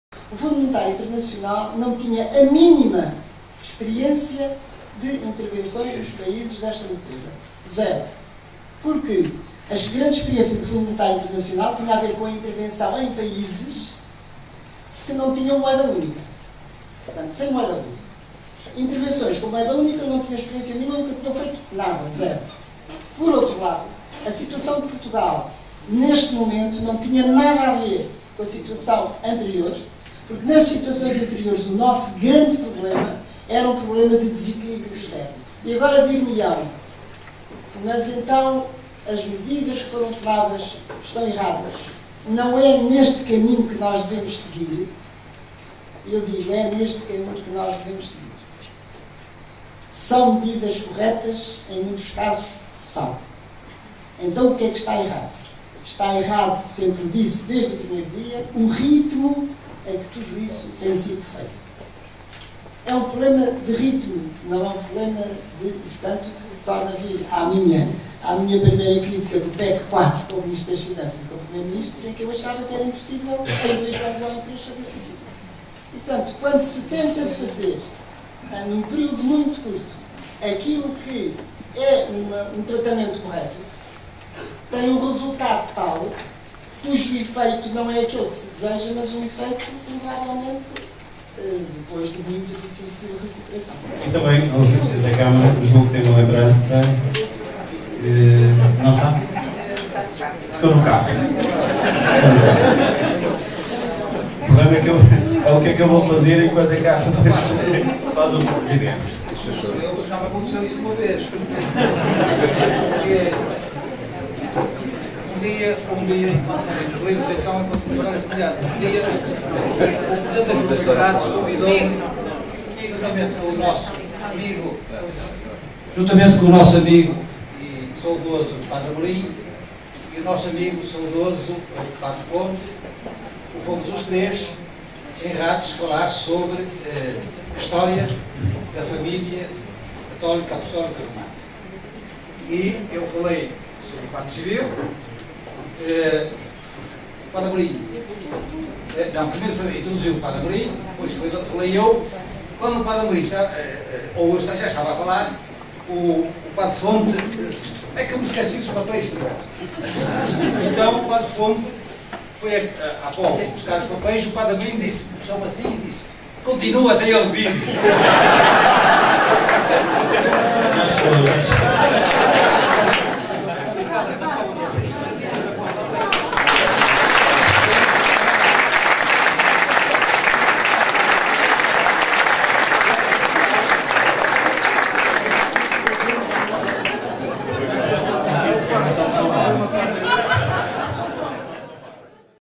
Manuela Ferreira Leite fala do Estado da Nação considerando-se realista
Manuela Ferreira Leite esteve no Clube de Caçadores da Estela, onde falou sobre "O Estado da Nação". A antiga líder do PSD abordou a atual intervenção da Troika em Portugal, a qual considera "um desastre", alertando para a possibilidade de Portugal vir a beneficiar do que se passa nos mercados centrais da Europa.